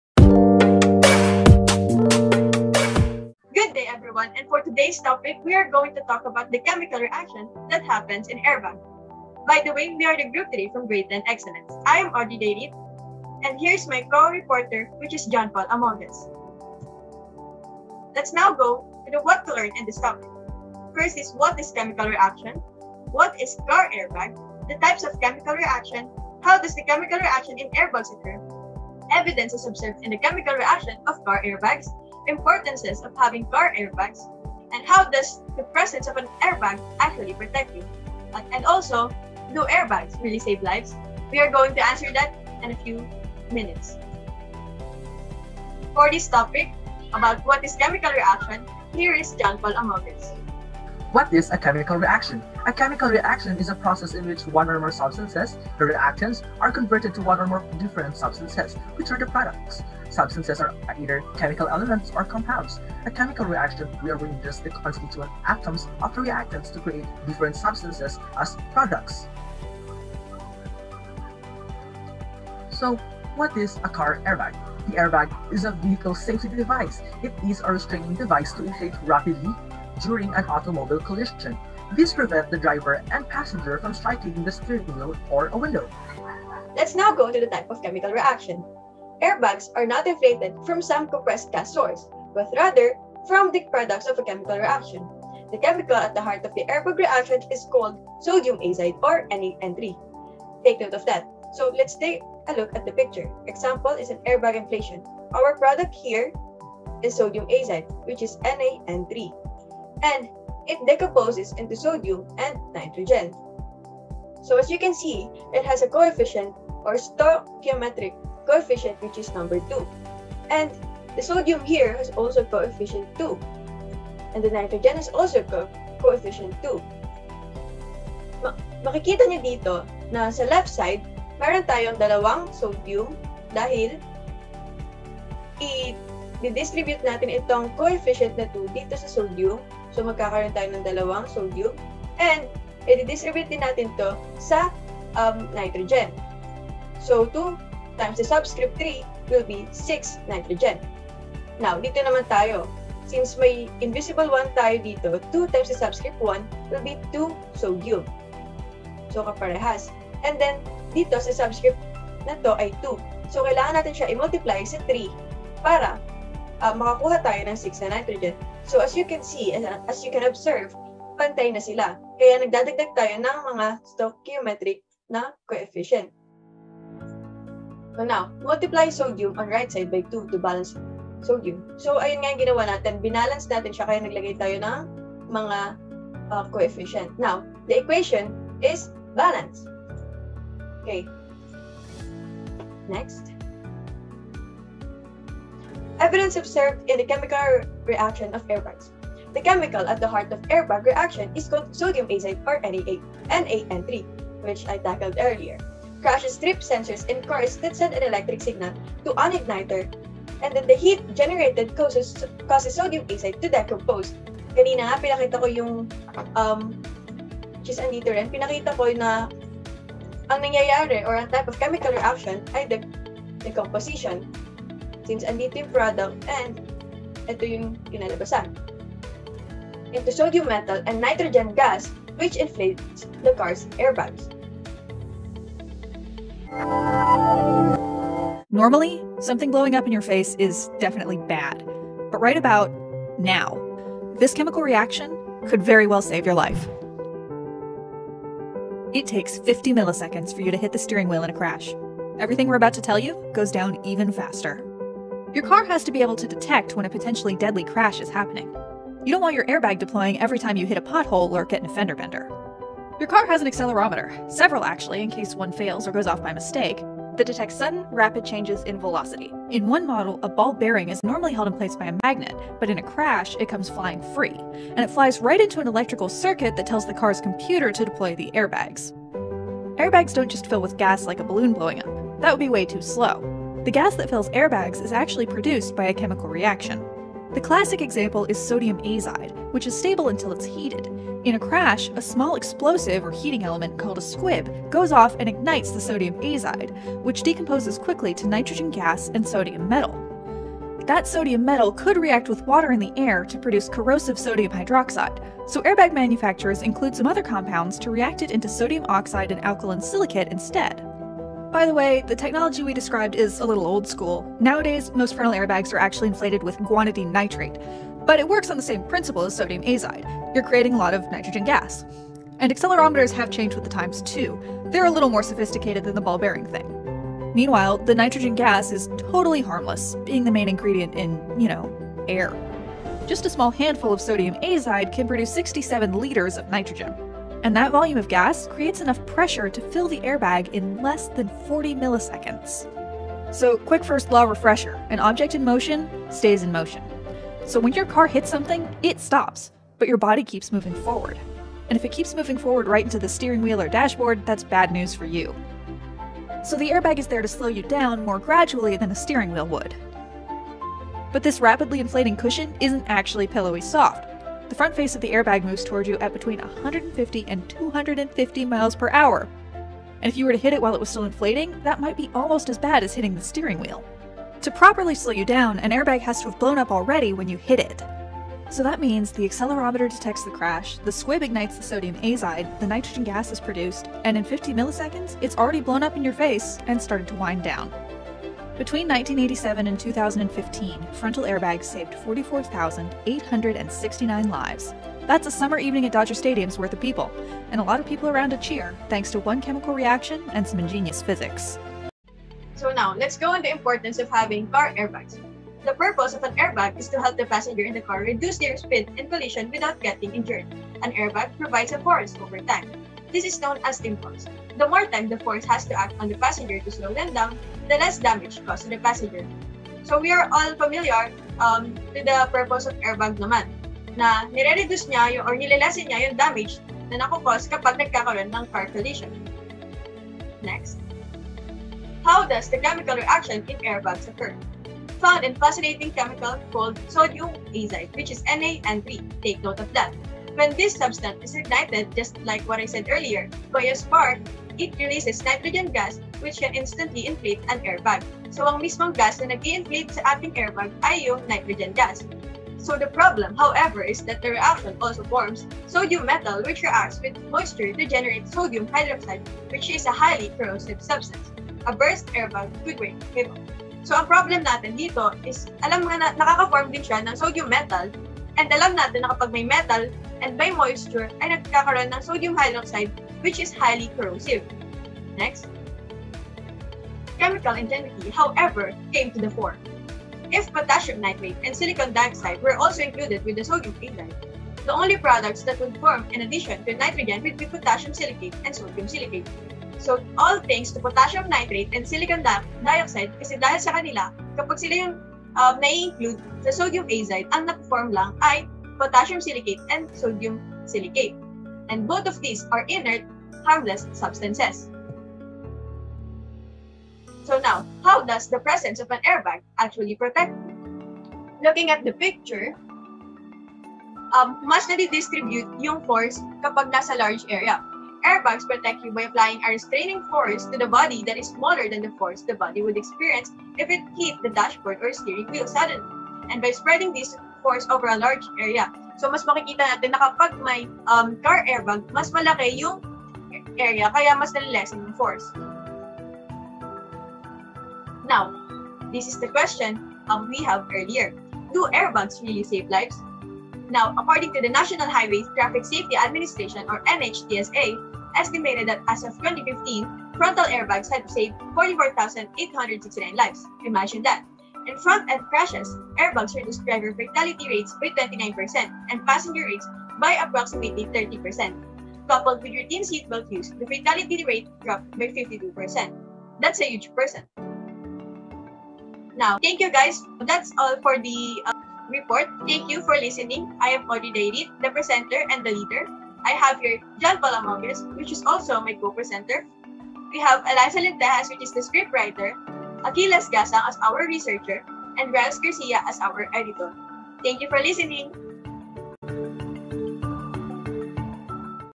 CHEMICAL REACTION THAT HAPPENS IN CAR AIRBAGS || GROUP 3 (EXCELLENCE)
Good day everyone! This video will give you information about the chemical reaction that happens in car airbag. It is for you to understand the topic further. Our group also discussed the chemical equation present in the reaction. Remember that we should always prioritize our safety. This is group 3 from 10-Excellence, thank you!